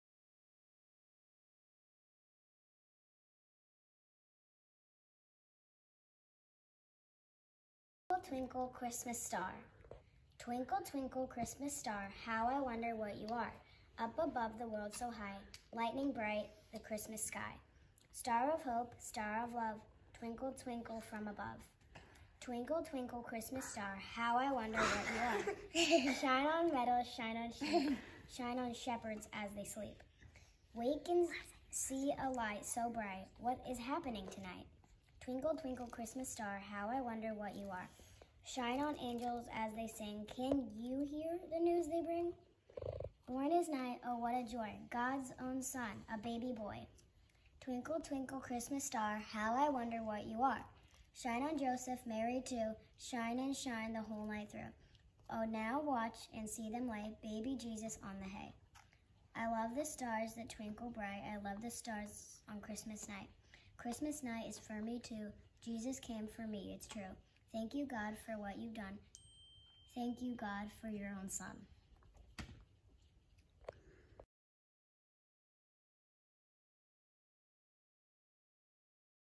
December 7th Advent Message - Twinkle, Twinkle Christmas Star
The Lundy Children read a Christmas story